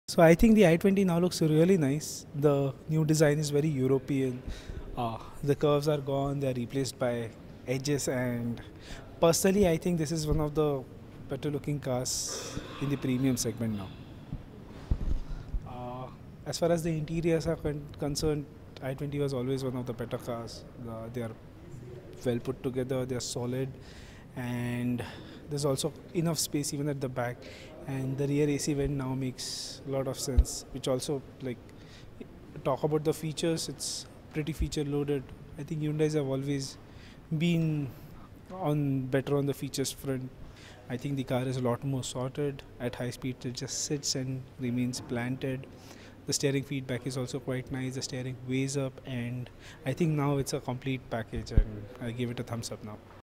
Hyundai | Elite i20 | Review | Carwale - Ninad Mirajgaonkar
Ninad Mirajgaonkar, Carwaale, feels that this is one of the better looking cars in the premium segment now. He says when it comes to the interior; the i20 always had the best. The car is well put together it is solid and there is ample space in the back. The car is fully feature loaded. He also says that Hyundai has always been on the front with respect to features. At high speed the car just remains planted to the ground, so there is stability.